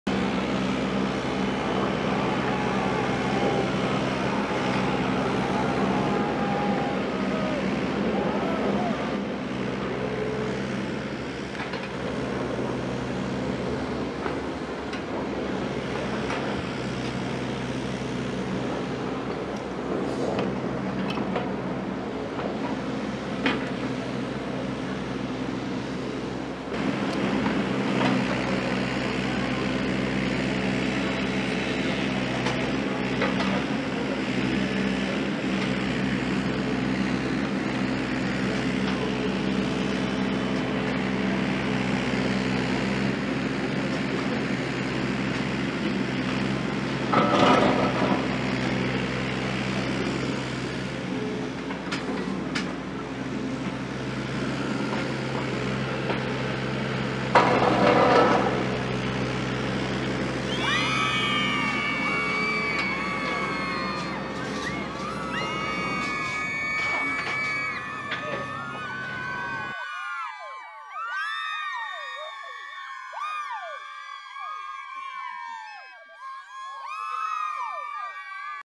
Bagger CAT 302.7D Archivvideo von der Abrissparty vom 15 Juni 18
Catella baut für Familien: 37 Townhouses mit Wohnungen um  100 qm auf zwei Ebenen, eigener Eingang, Vorgarten und Garten. Sie werden ab 1000 Euro Kaltmiete zu haben sein. Zwei Kindergärten mit je 75 Plätzen sind geplant Altergerechtes wohnen mit Wohngruppe werden realisiert mit Gemeinschaftsküchen oder Gesellschaftsräume. Catella sieht auch betreutes Wohnen vor. Das Fachpersonal wohnt auch im Komplex. Aber jetzt wird das alte Post Verteilzentrum erst mal von Laarakkers abgerissen